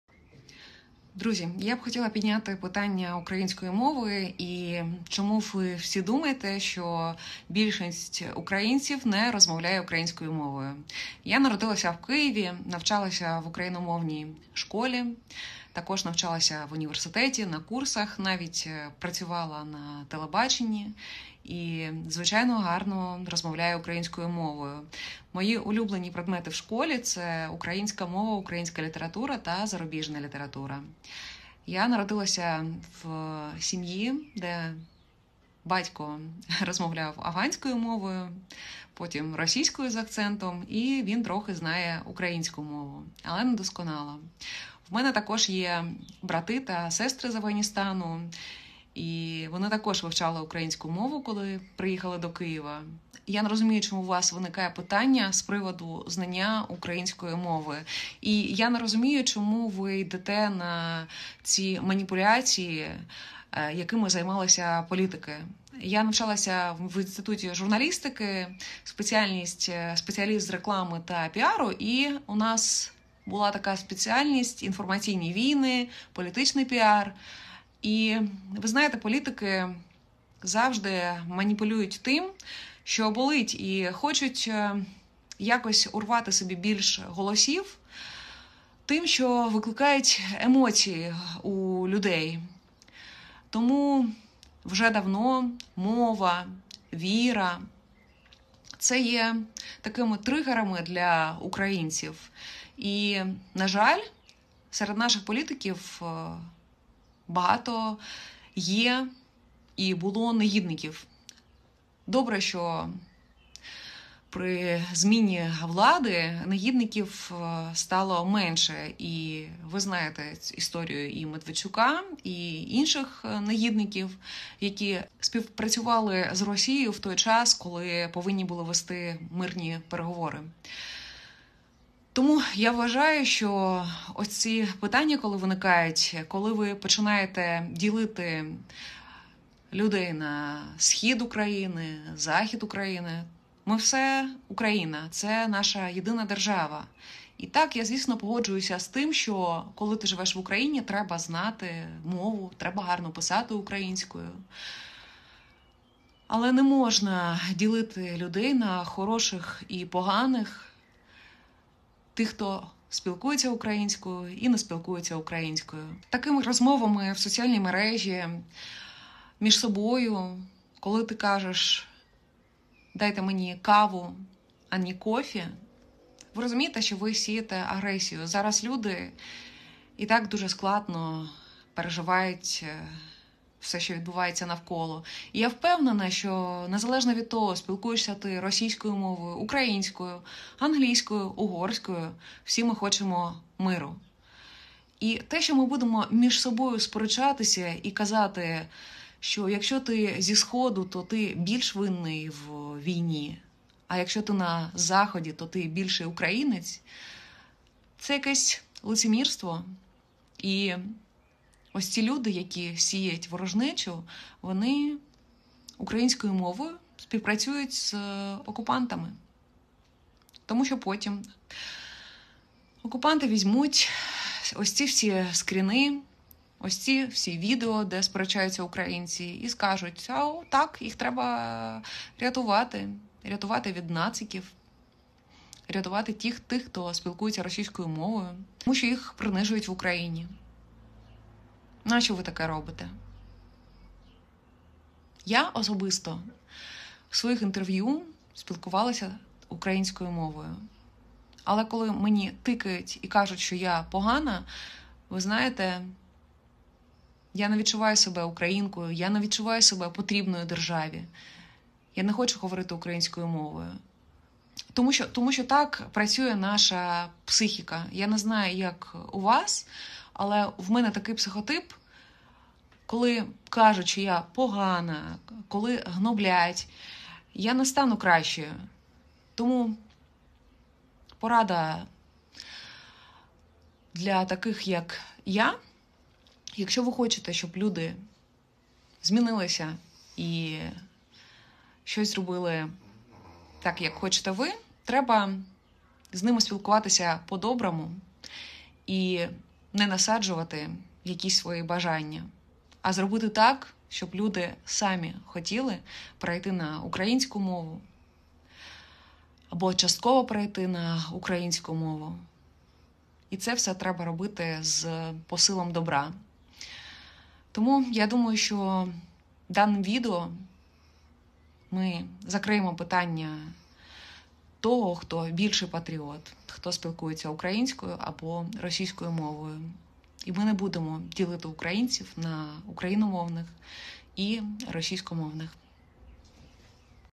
Раміна заявила, що не хоче спілкуватися українською, і закликала не нав'язувати їй свою думку
Рамина заявила, что не хочет общаться на украинском, и призвала не навязывать ей свое мнение
Ramina stated that she did not want to speak Ukrainian and urged not to impose her opinion on her